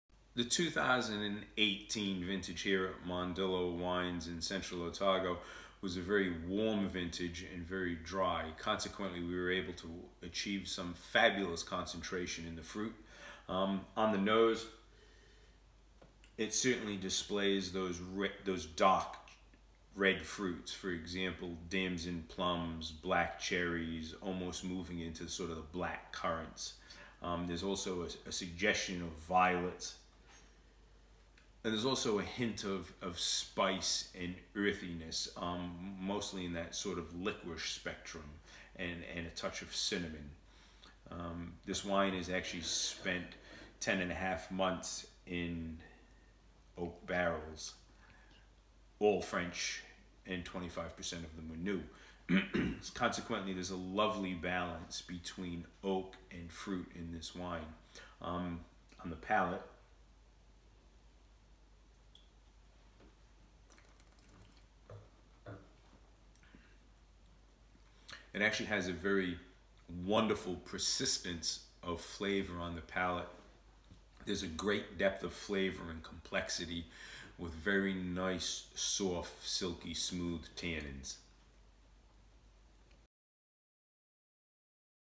Taste the 2018 Mondillo Pinot Noir with Winemaker Dominic Mondillo
2018 was warm and dry in Central Otago resulting in fantastic fruit concentration in the Mondillo Pinot Noir.